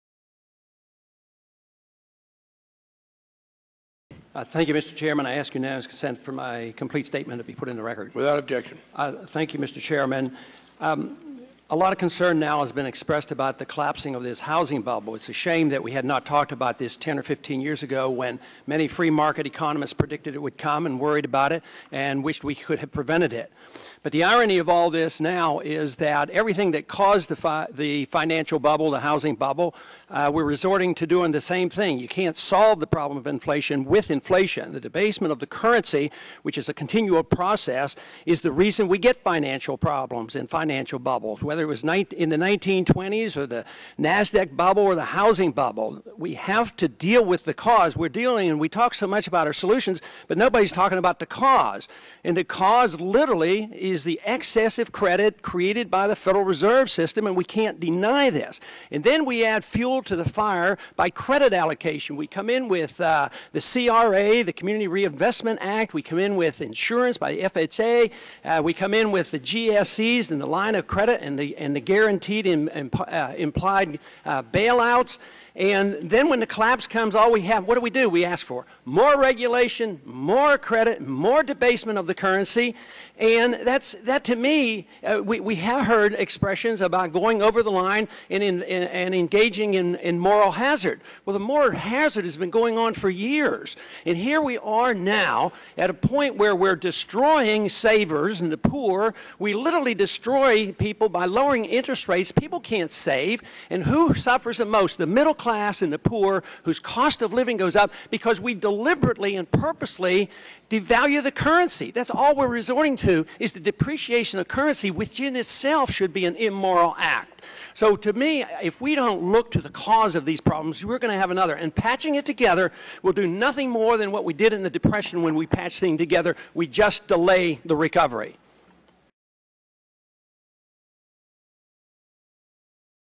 FORECLOSURES & THE ECONOMY: Rep. Ron Paul
Video from:

House Financial Services Committee - September 20, 2007

Full committee hearing on "legislative and regulatory options for minimizing
and mitigating mortgage foreclosures."  Witnesses included Henry Paulson, Secretary of the Treasury; Alphonso Jackson, Secretary of Housing and Urban
Development; and Ben Bernanke, Chairman, Board of Governors of the Federal Reserve System.